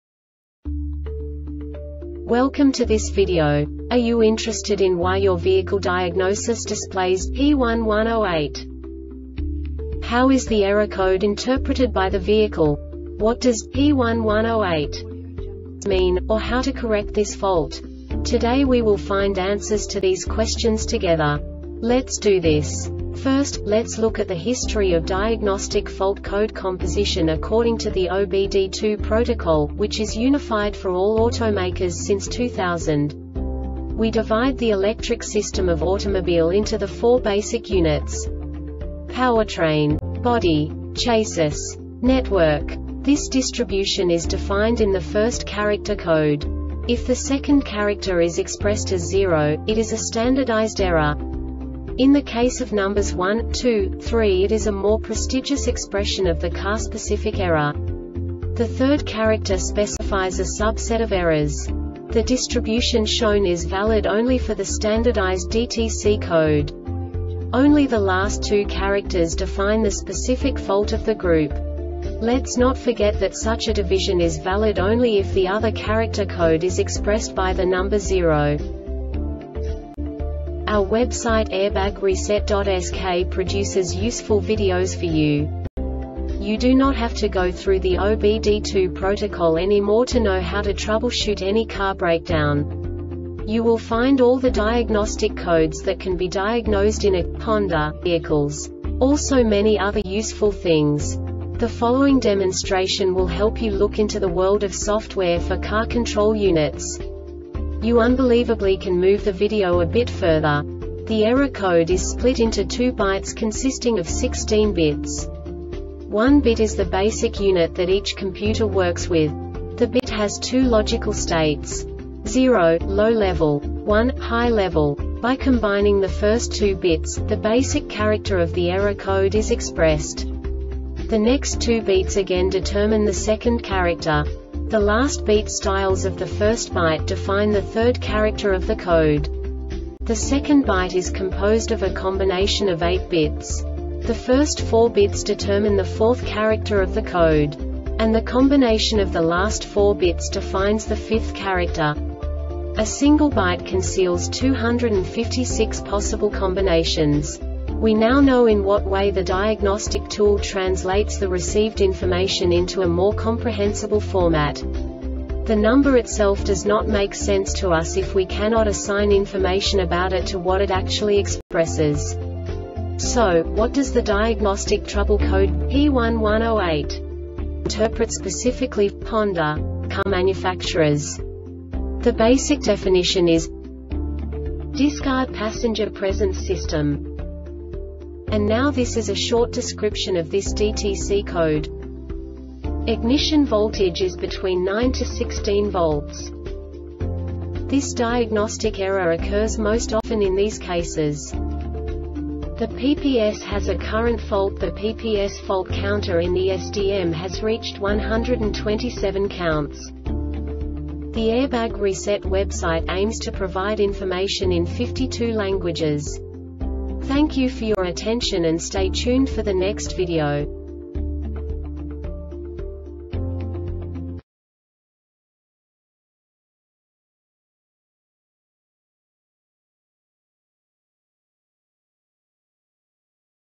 DTC Honda P1108 Short Explanation
Contents:
0:21 Basic DTC analysis according to OBD2 protocol standard.
1:48 Insight into programming
2:58 A diagnostically understandable description of the Diagnostic Trouble Code
3:05 Basic error definition

Make:
Honda
Code:
P1108
Definition:
Discard Passenger Presence System 1
Description:
Ignition voltage is between 9-16 volts.
Cause:
The PPS has a current fault.The PPS fault counter in the SDM has reached 127 counts.